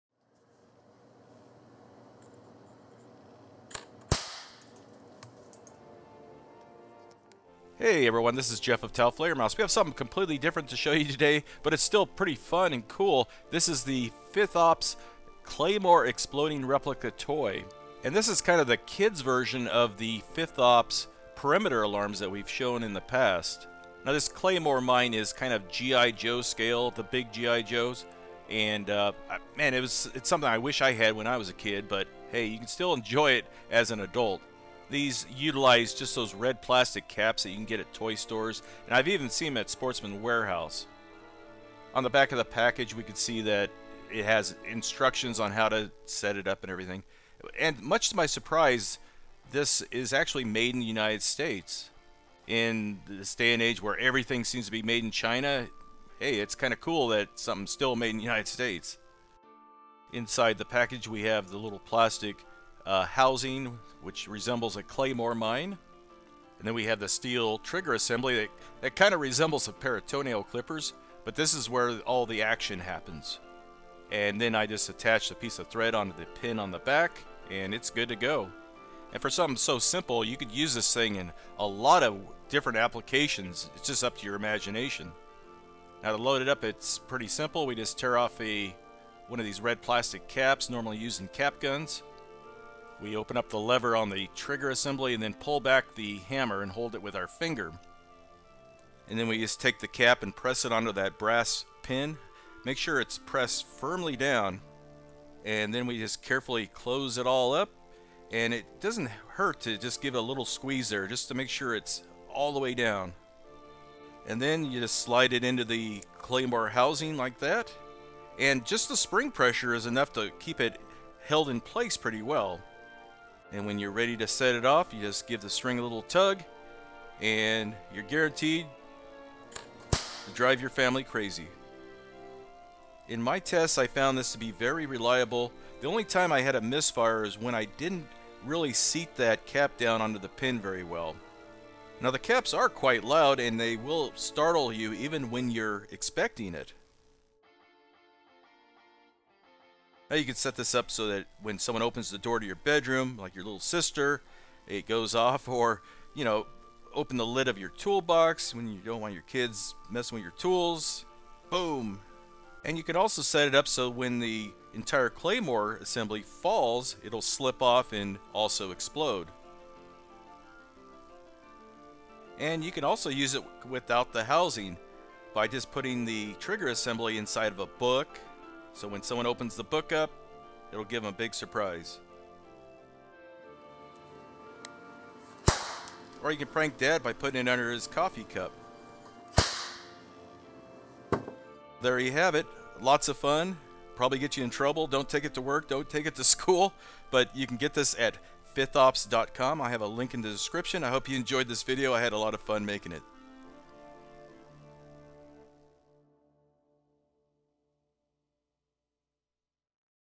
EXPLODING Claymore Mine Toy - It's a real thing!
We review and demonstrate the new Fithops exploding claymore replica toy.  It is safe for kids 8 and beyond.    Surprisingly  it is made in the USA and is well-constructed.

This is an unsponsored review.